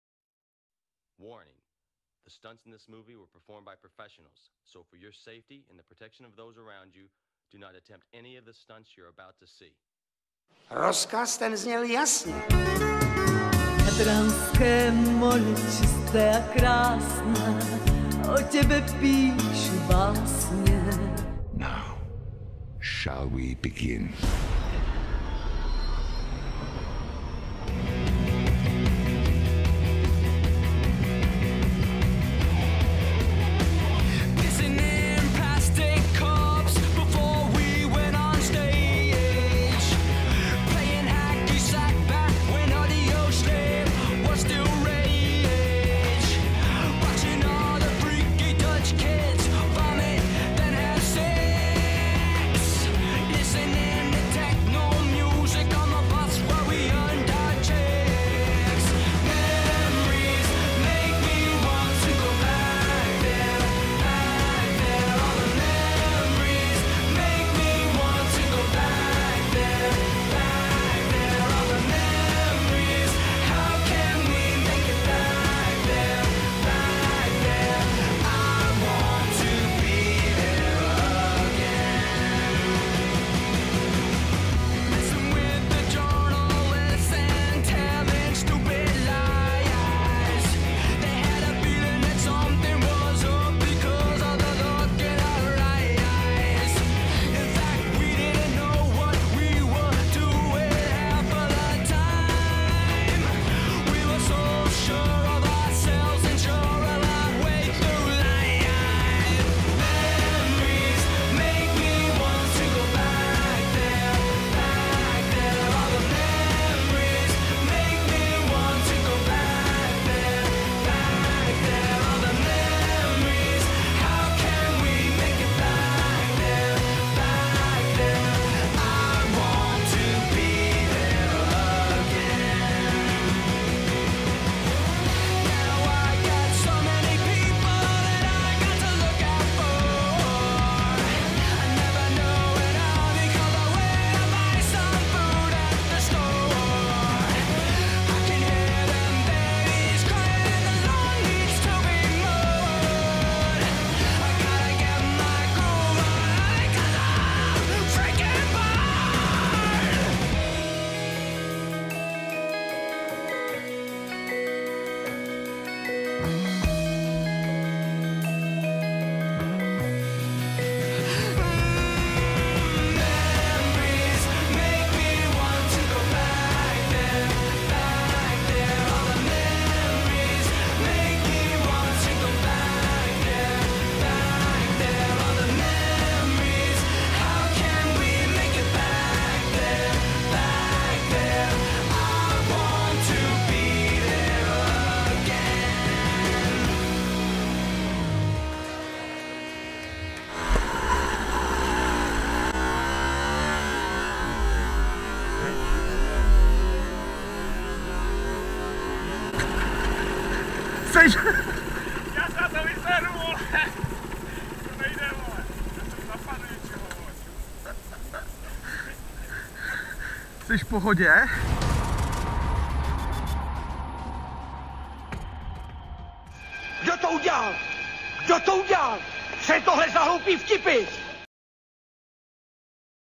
Na fichtlech do Chorvatska 2016
Zatím krátká ukázka naší jizdy do Chorvatska, než se sestříhá celý film z této výpravy. Pokud se vám naše cesty líbí, můžete se přihlásit i k našemu facebookovému profilu ,nebo mrknout na fotky na rajčeti